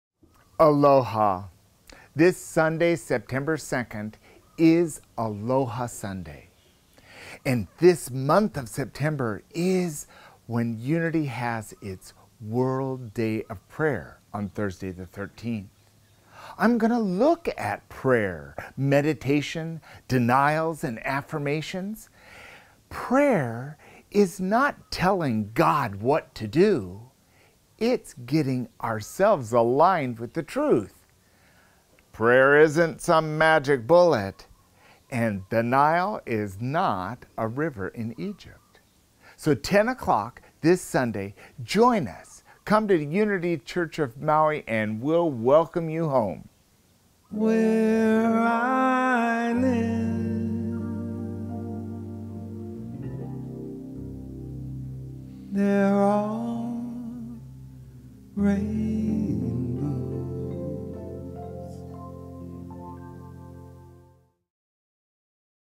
Next Sunday, Rev. Gary Canier, "Practicing the Presence"
Rev. Gary will celebrate Aloha Sunday and Unity's World Day of Prayer with a talk on what really is prayer and meditation, and how we can align ourselves with these powerful practices. Sunday, September 2 at 10:00am, Unity Maui, 483 S. High St., Wailuku.